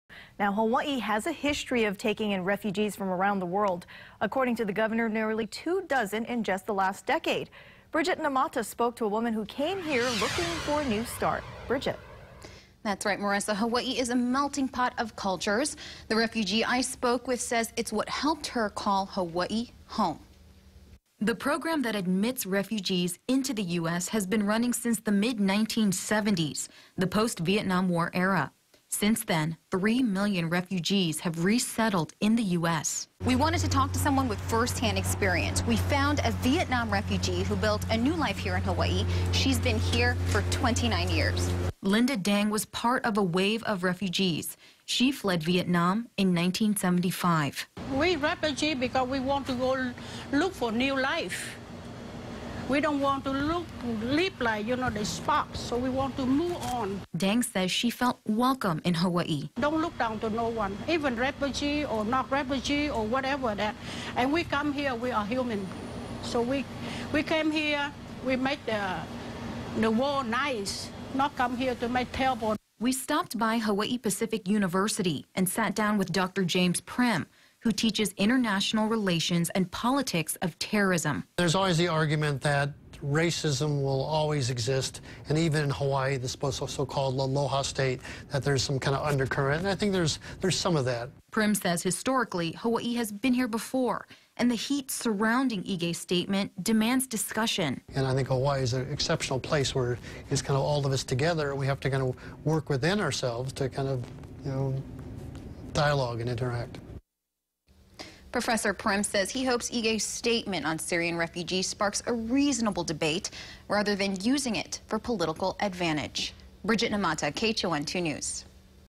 Vietnamese refugee felt welcome in Hawaii
Oahu resident Linda Dang was part of a wave of refugees who fled Vietnam in the 1970s.